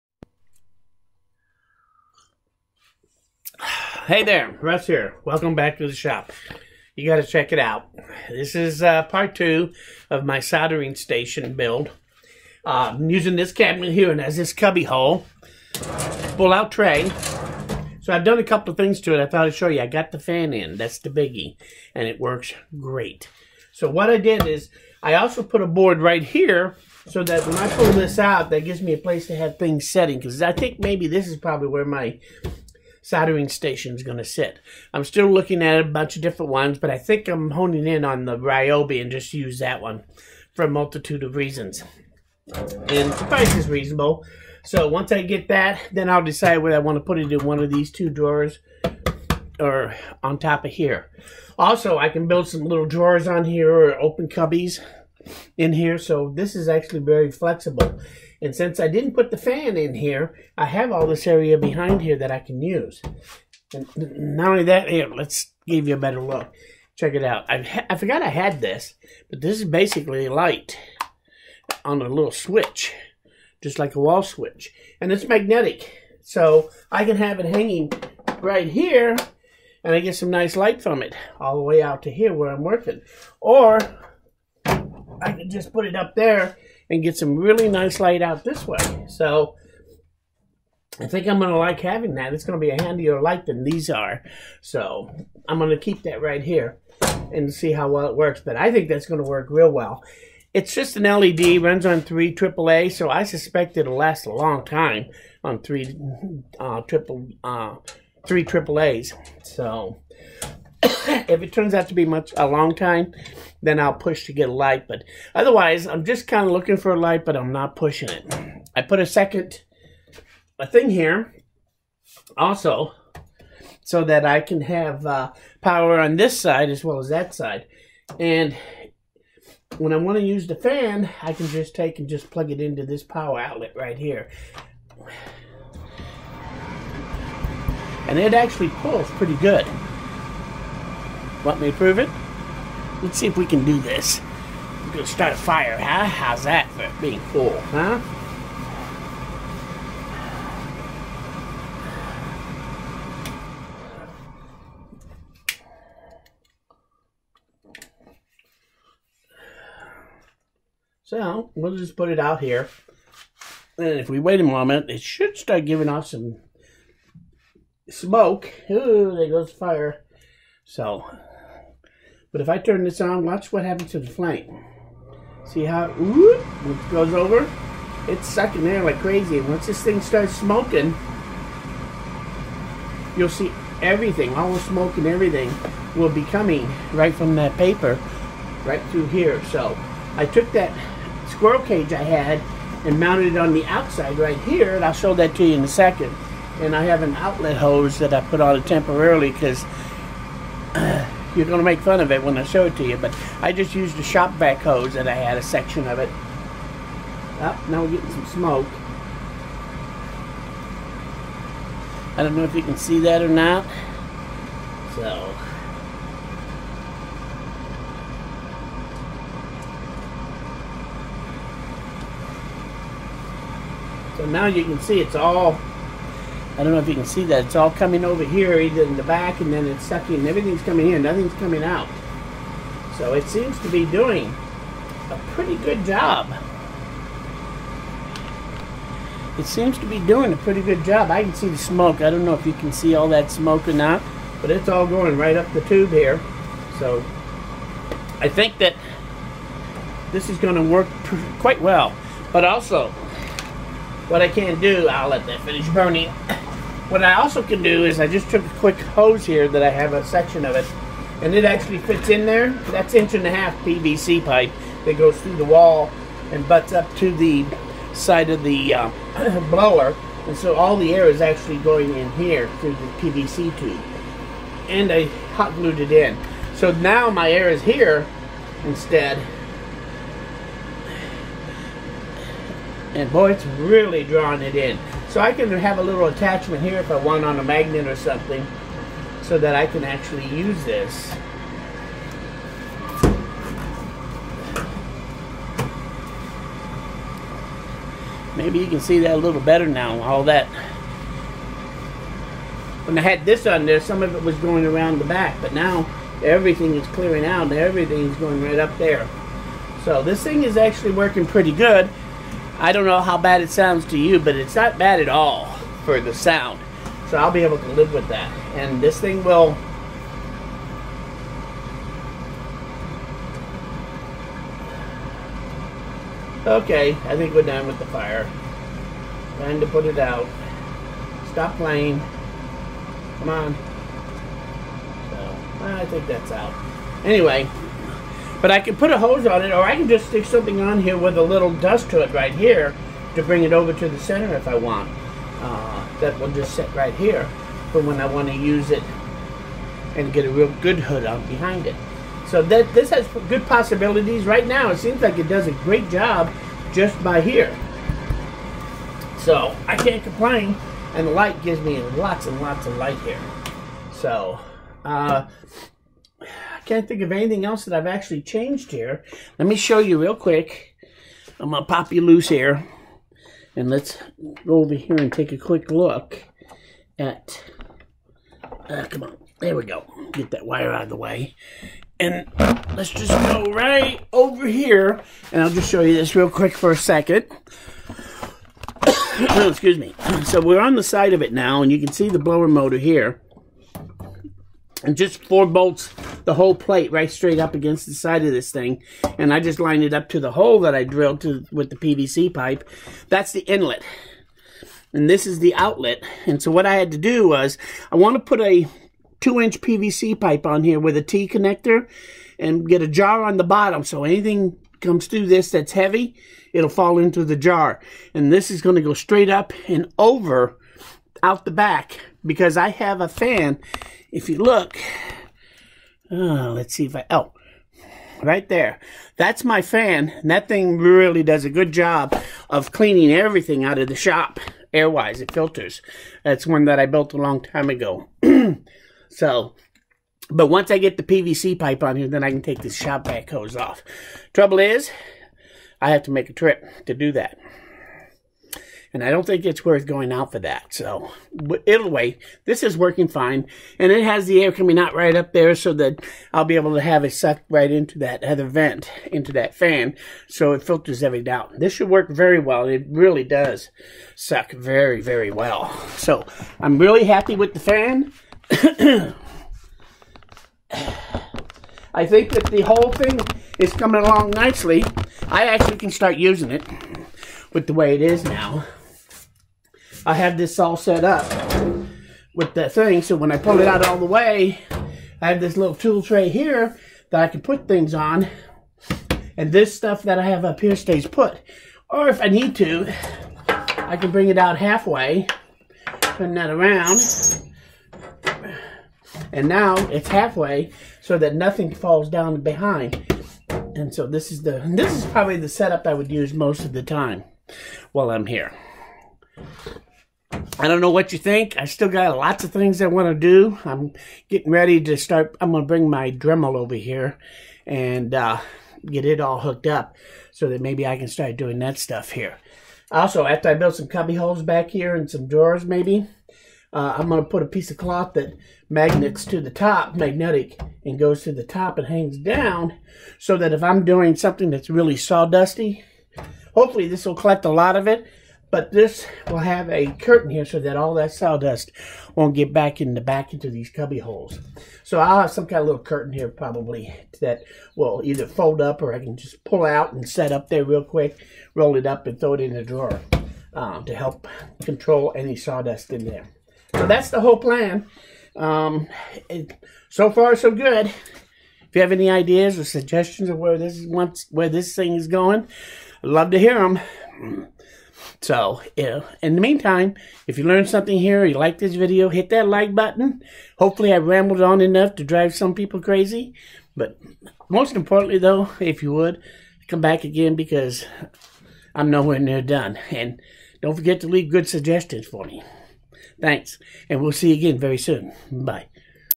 Soldering Station Fumes Extraction by Rambling Russ
Rambling Russ.  Soldering Station Part 2.  Check out how I installed a fan on my soldering station and see how well it works.  Also, a couple of other quick things that I have added.  The soldering station is slowly coming together.